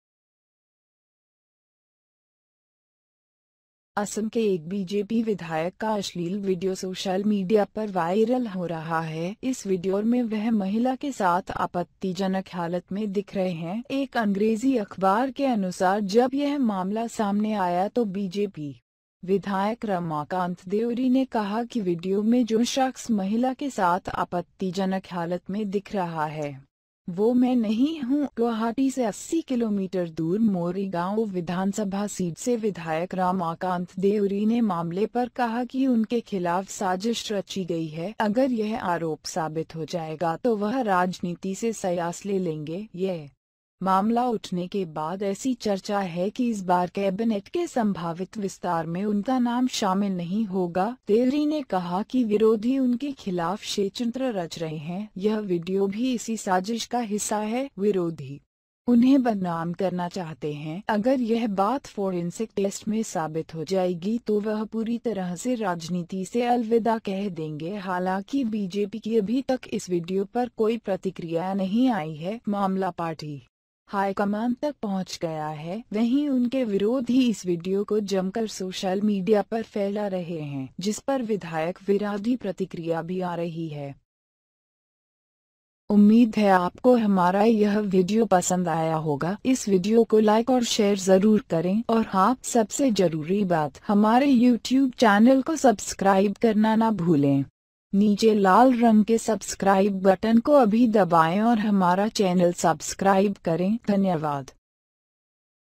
Assam के BJP MLA Ramakanta Dewri Sex-Scandal में फंसे, एक महिला संग अश्लील Video हुआ Viral..
असम के BJP विधायक रमाकांत देवरी सेक्स-स्कैंडल में फंसे, एक महिला संग अश्लील विडियो हुआ Viral.
BJP MLA from Assam, Ramakanta Dewri has been found involved in a sex-scandal. Recently, a video in which Ramakanta can been seen in a compromising position with a woman has gone viral on social media.
असम के एक BJP विधायक का अश्‍लील वीडियो सोशल मीडिया पर Viral हो रहा है। इस वीडियो में वह महिला के साथ आपत्तिजनक हालत में दिख रहे हैं। एक अंग्रेजी अखबार के अनुसार जब यह मामला सामने आया तो BJP विधायक रमाकांत देउरी ने कहा कि वीडियो में जो शख्‍स महिला के साथ आपत्तिजनक हालत में दिख रहा है, वो मैं नहीं हूं।
गुवाहाटी से 80 किलोमीटर दूर मोरीगांव विधानसभा सीट से विधायक रमाकांत देउरी ने मामले पर कहा कि उनके खिलाफ साजिश रची गई है। अगर यह आरोप साबित हो जाएगा तो वह राजनीति से संयास ले लेंगे। यह मामला उठने के बाद ऐसी चर्चा है कि इस बार कैबिनेट के संभावित विस्‍तार में उनका नाम शामिल नहीं होगा। देउरी ने कहा कि विरोधी उनके खिलाफ षयचंत्र रच रहे हैं। यह वीडियो भी इसी साजिश का हिस्‍सा है। विरोधी उन्‍हें बदनाम करना चाहते हैं। अगर यह बात फोरेंसिक टेस्‍ट में साबित हो जाएगी तो वह पूरी तरह से राजनीति से अलविदा कह देंगे
हालांकि BJP की अभी तक इस वीडियो पर कोई प्रतिक्रिया नहीं आई है। मामला पार्टी हाईकमान तक पहुंच गया है। वहीं उनके विरोधी इस वीडियो को जमकर सोशल मीडिया पर फैला रहे हैं। जिसपर विधायक विराधी प्रतिक्रिया भी आ रही है।

BJPLatestNewsinHindi.